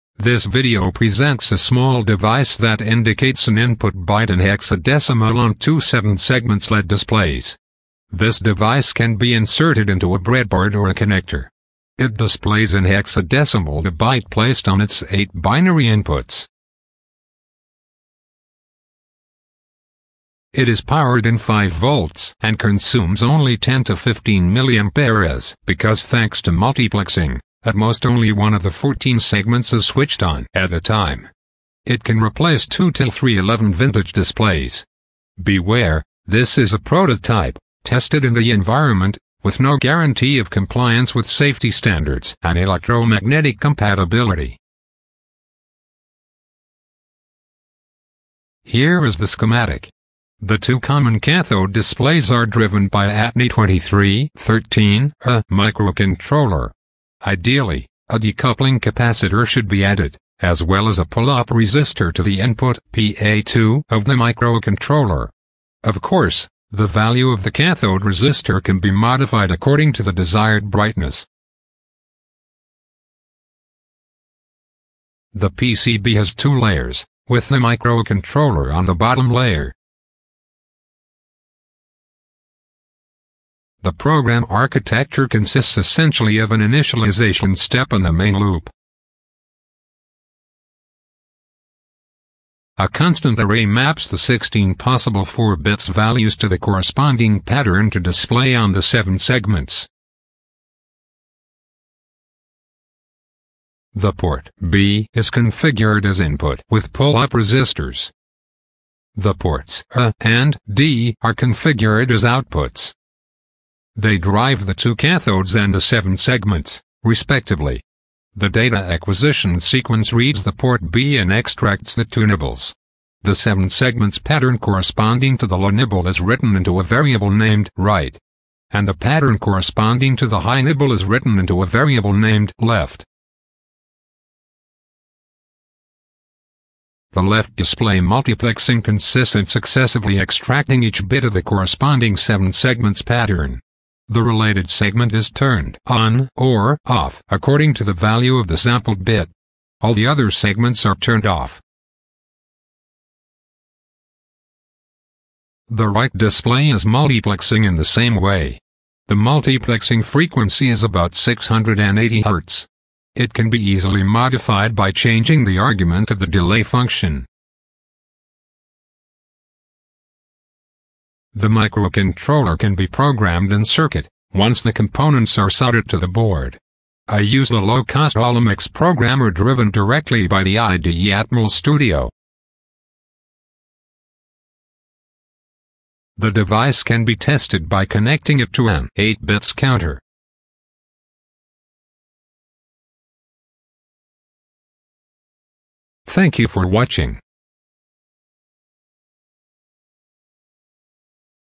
Hexadecimal display device
This video presents a small device that indicates an input byte in hexadecimal on two 7 segments led displays. This device can be inserted into a breadboard or a connector. It displays in hexadecimal the byte placed on its 8 binary inputs.

You can access to the PCB design and the schematic